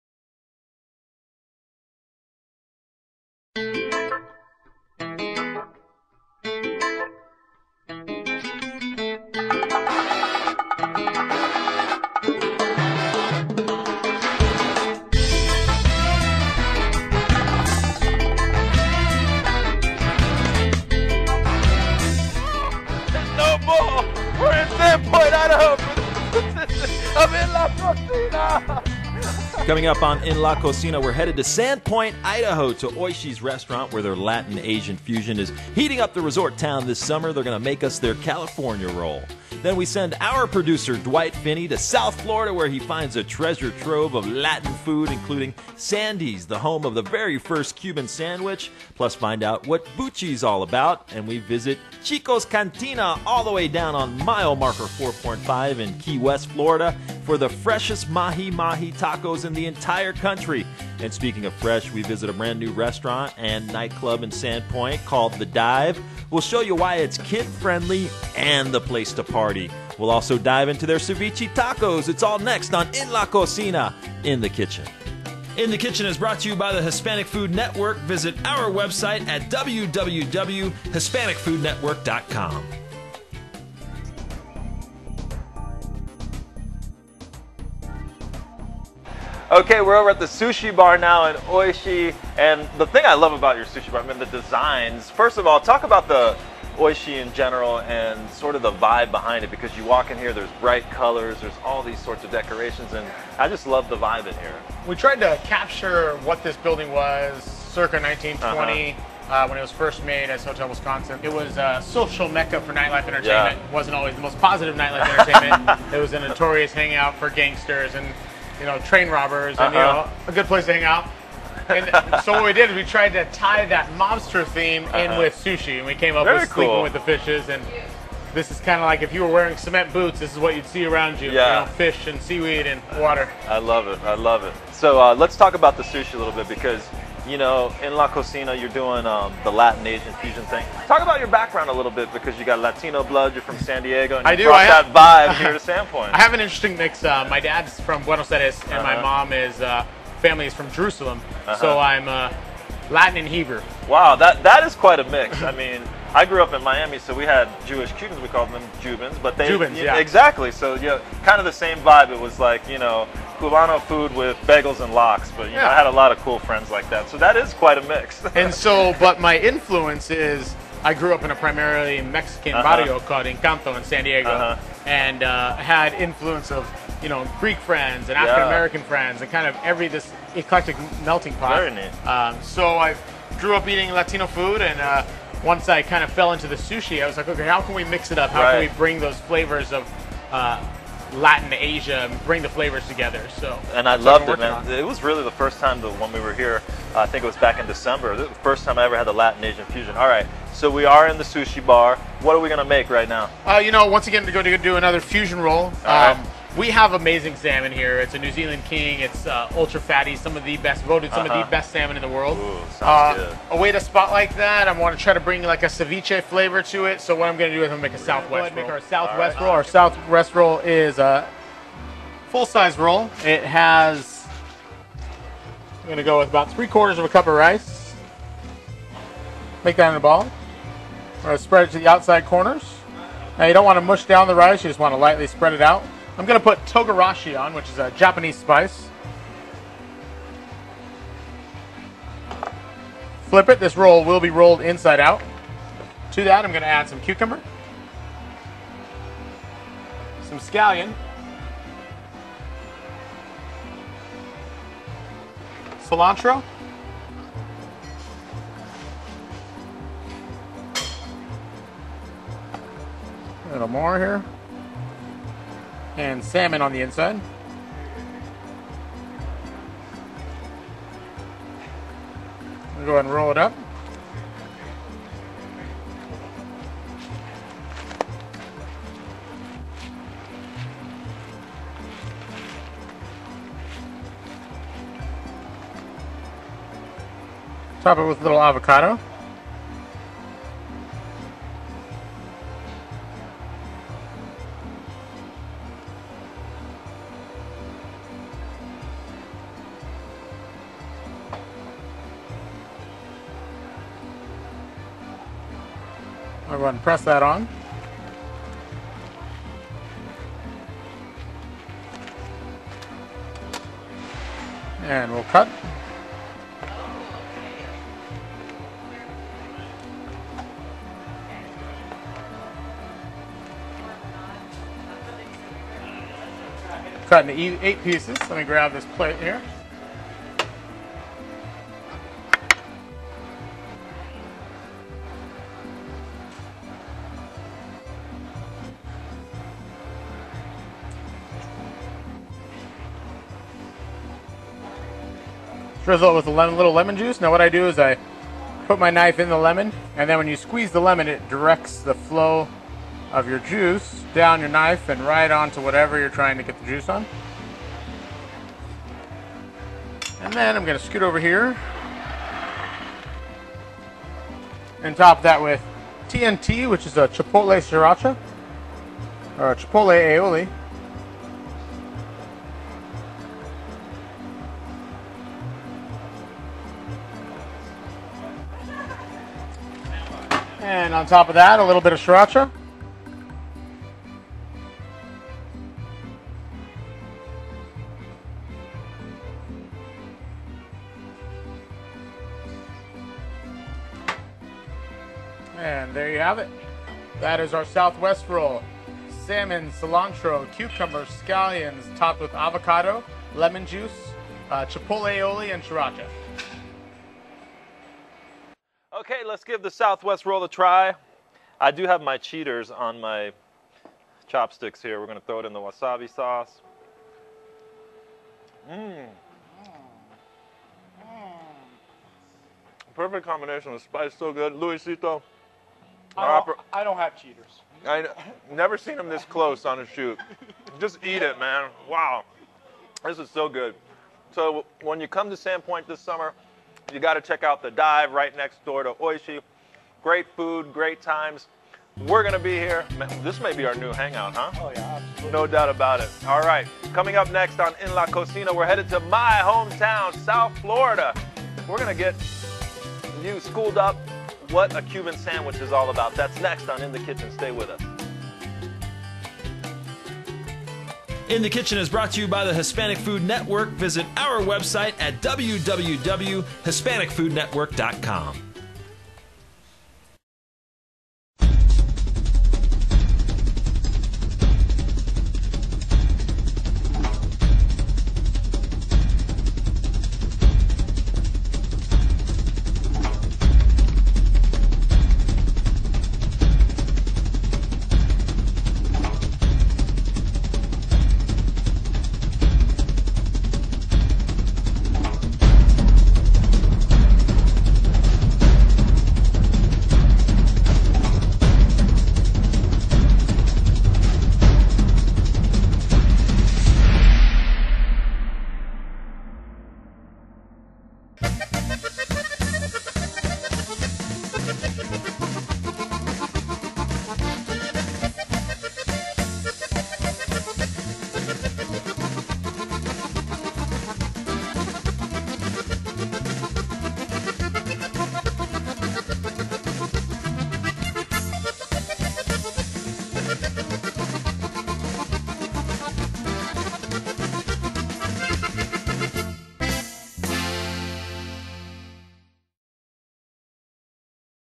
Oishii In Sandpoint, Id
Part one of our Coast to Coast Show! Check out Oishii in Sanpoint Idaho.